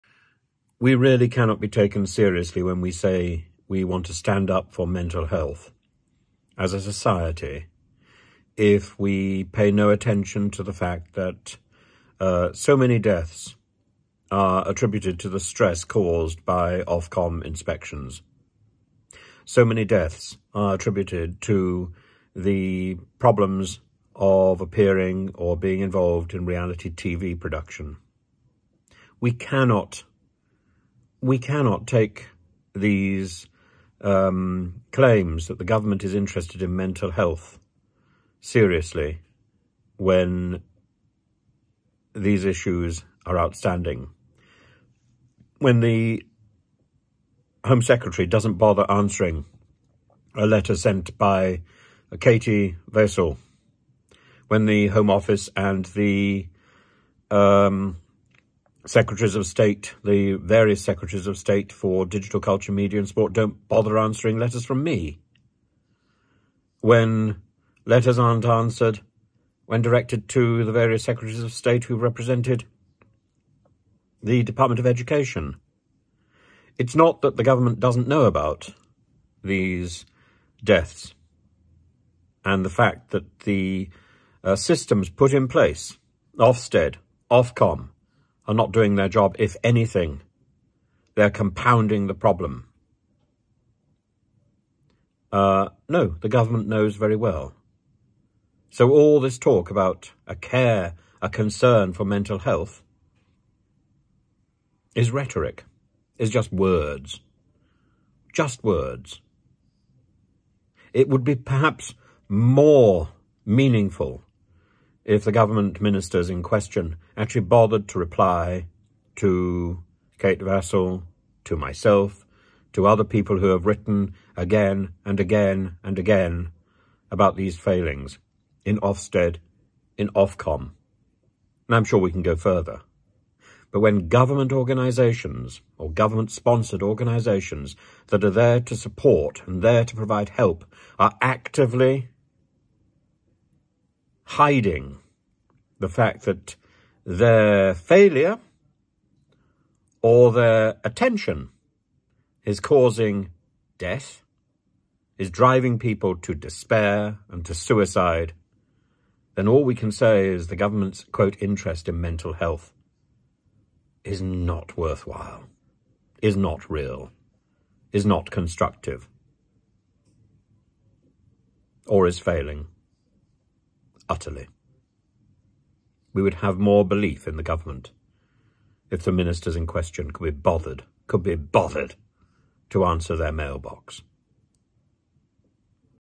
As the guardian records the coroners verdict that Ofsted inspections have led to numerous deaths
And as ofcom fails to act to constructively reform reality tv or for its ministers of education and DCMS to answer mail, we must conclude that the claims to care about mental health are window dressing. Hot air and rhetoric to disguise indolence and indifference across  various ministry’s and disciplines